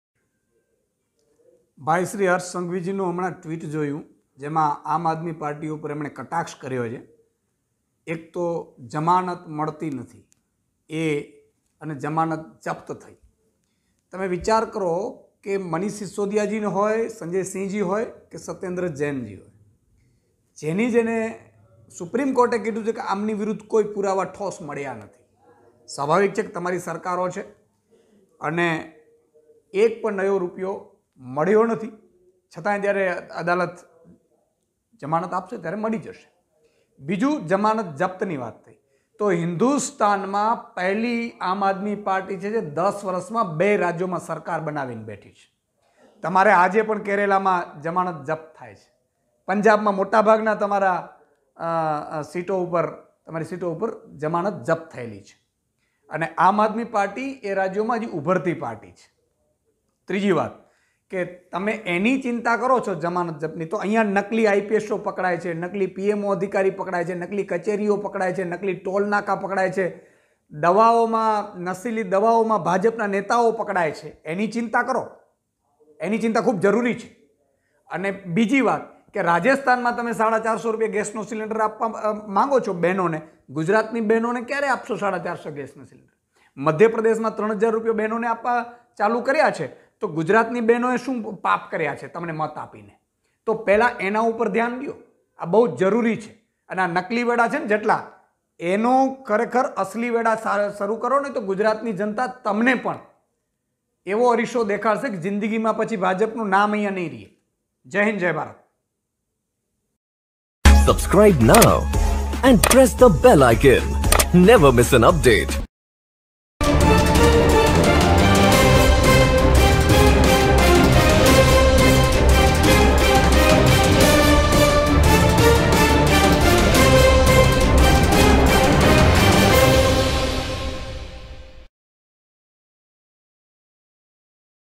Isudan Gadhvi: ગુજરાતમાં નકલી ચીજ વસ્તુઓનો રાફડો ફાટી નિકળ્યો છે! | Aap Gujarat | Dpnews
ગુજરાત: નકલી ચીજ વસ્તુઓનો રાફડો ફાટી નિકળ્યો છે!
ગુજરાતમાં નકલી ચીજ વસ્તુઓનો રાફડો ફાટી નિકળ્યો છે